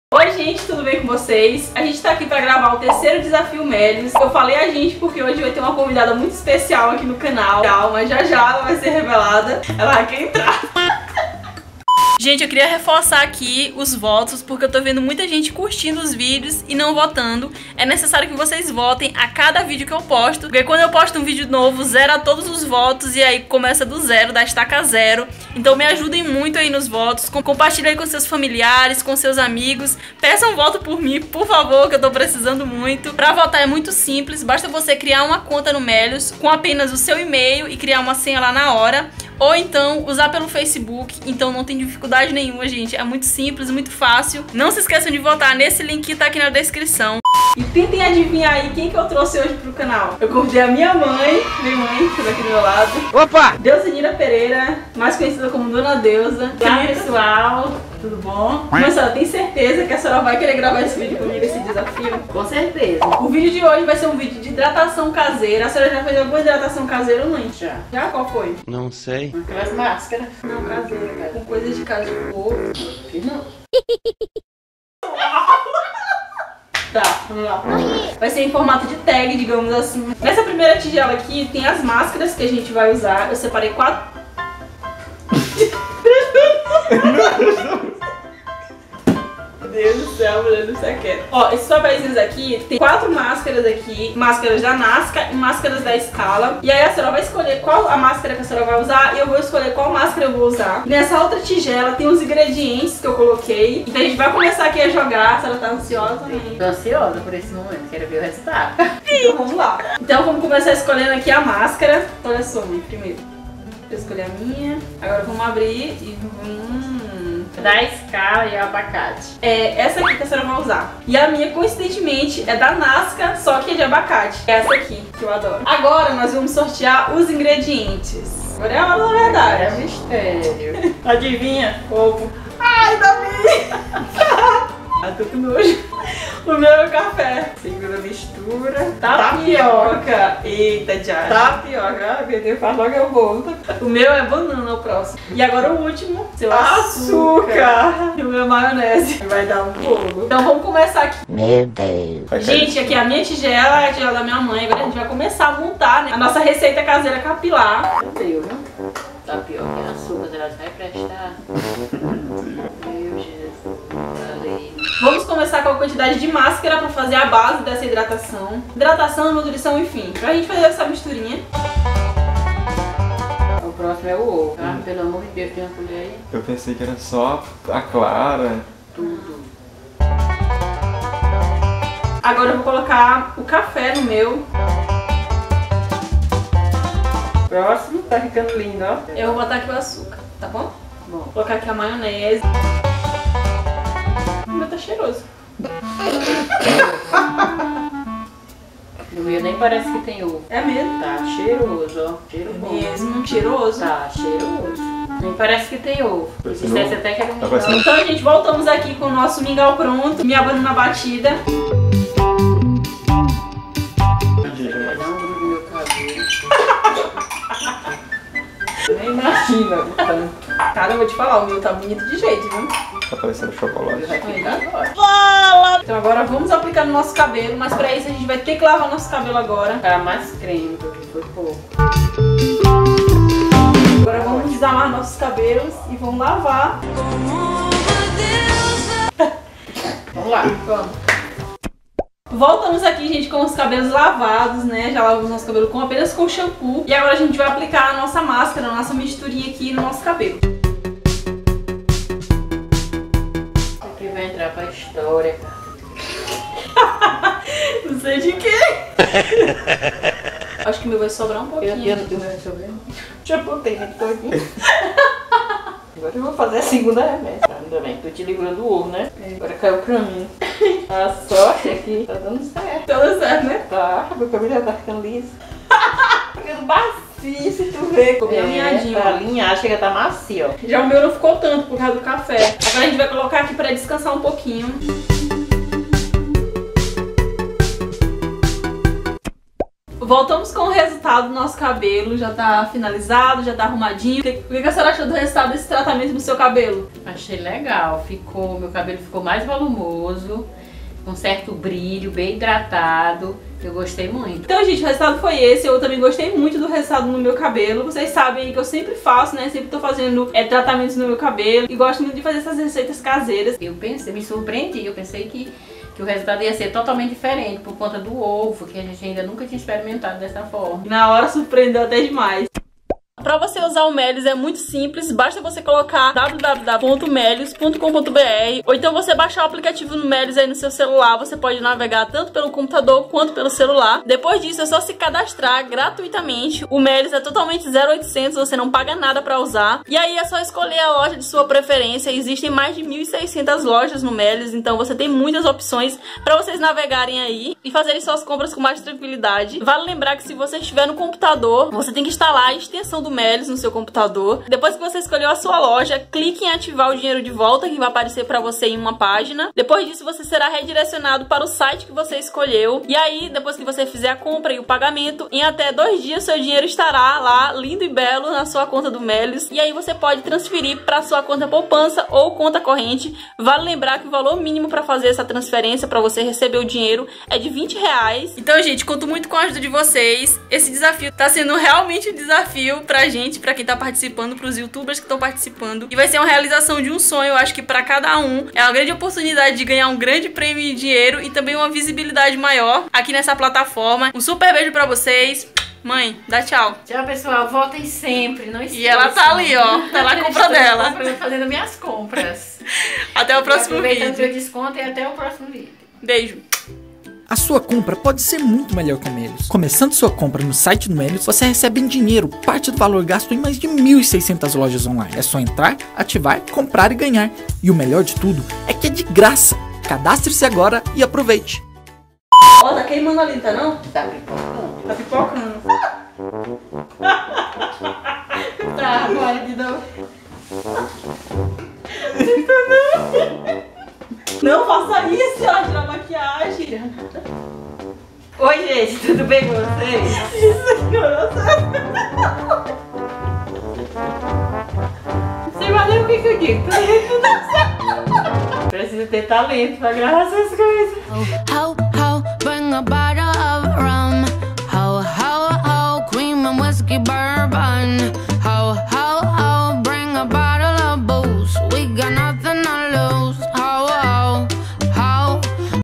RECEITA CASEIRA mais LOUCA da INTERNET (resultado incrível) - CACHEADA X ONDULADA - Desafio Meliuz 2
*Erros de gravação no final do vídeo.

Edição: Carmem Batista